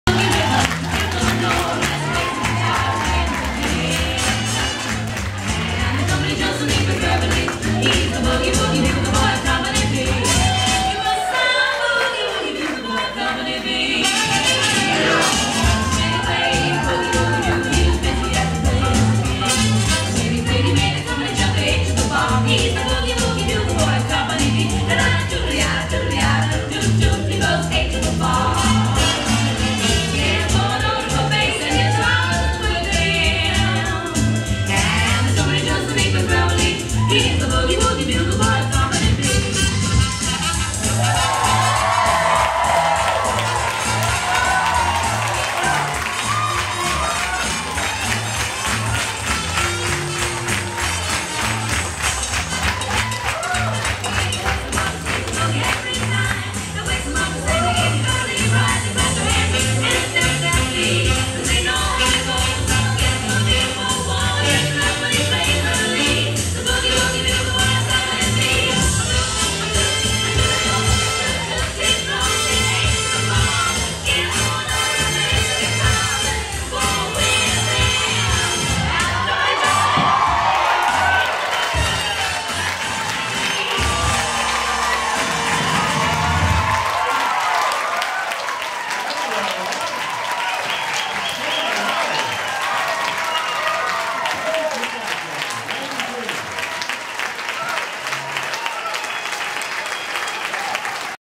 "Now dancing the Jive...Wade and his partner, Julie"
This dance brought the crowd to their feet at "Hospice PEI presents Dancing with the Stars", October 13, 2012. You two rocked it!!!